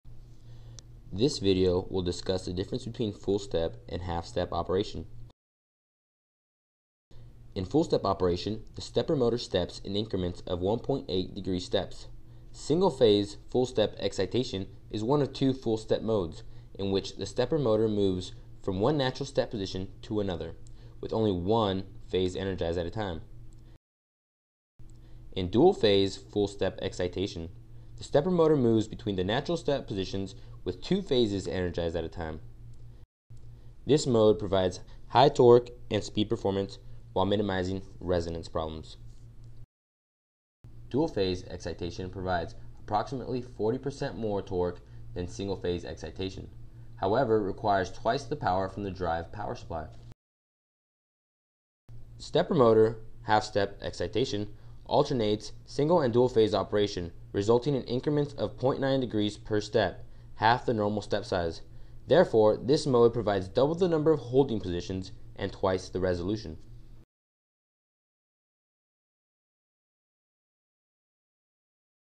Stepper Motors: What is the Difference Between Full-Step and Half-Step Modes?
This video will detail the similarities and differences between full and half-step modes in regards to running a stepper motor. for a large selection of high-quality stepper motor products, at low prices.